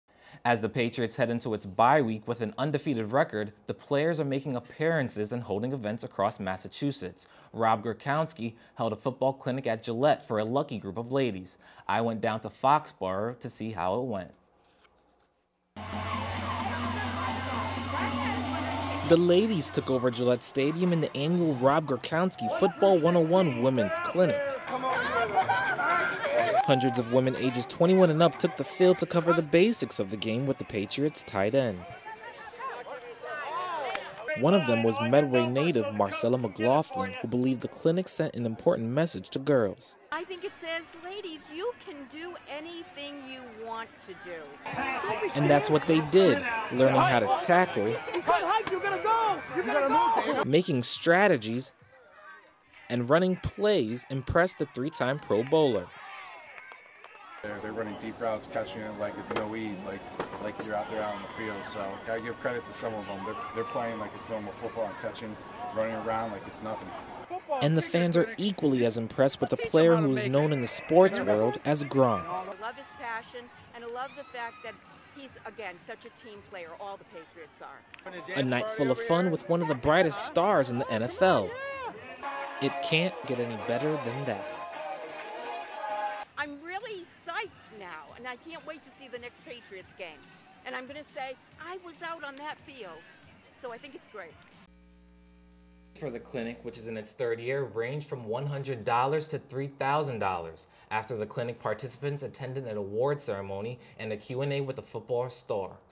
Rob Gronkowski's Football 101 Women's Clinic
During the Patriots' bye week, tight end Rob Gronkowski held a football 101 clinic just for the ladies. Brandon Lewis took a trip to Foxboro.